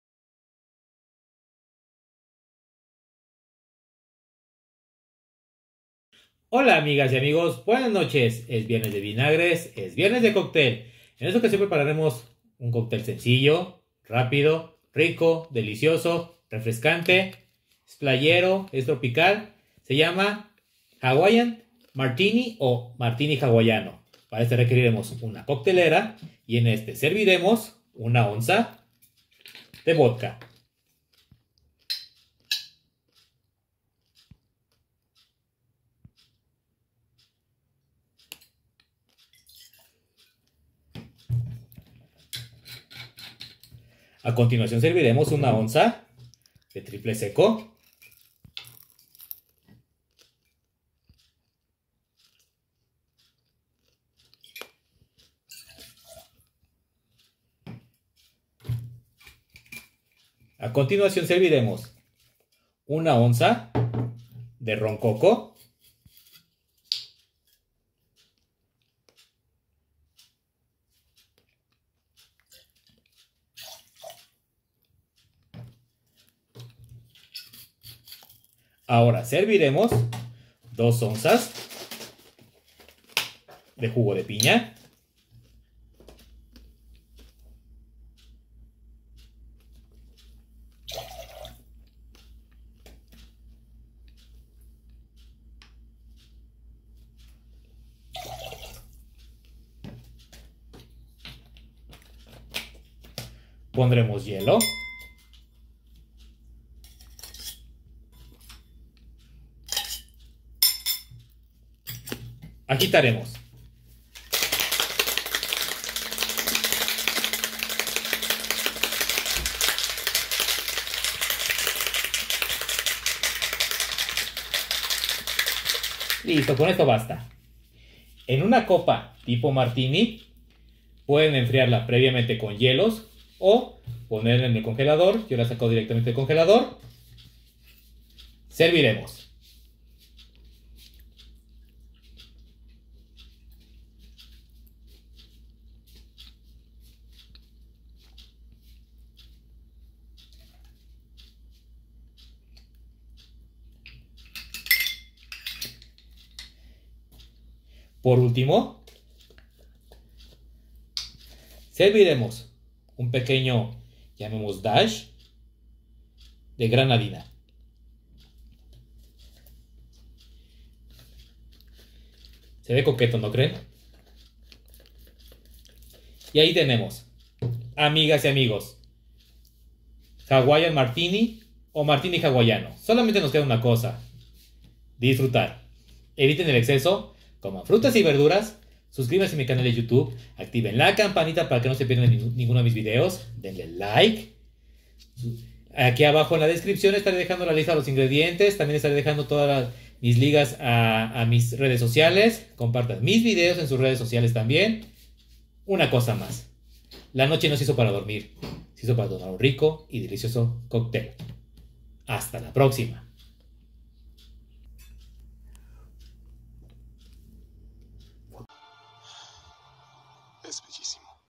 Como preparar Cóctel Hawaiian Martini
Ingredientes:

- 1 onza o 30 ml de vodka
- 1 onza o 30 ml de ron blanco
- 1 onza o 30 ml de triple sec
- 2 onzas o 60 ml de jugo de piña
- 2 dashes de granadina

Usar copa tipo martini

SIGUEME TAMBIÉN EN INSTAGRAM DONDE ESTARÉ SUBIENDO IMÁGENES DE ESTE CÓCTEL

COMPARTE MIS VÍDEOS 😃 CON TOD@S TUS AMIG@S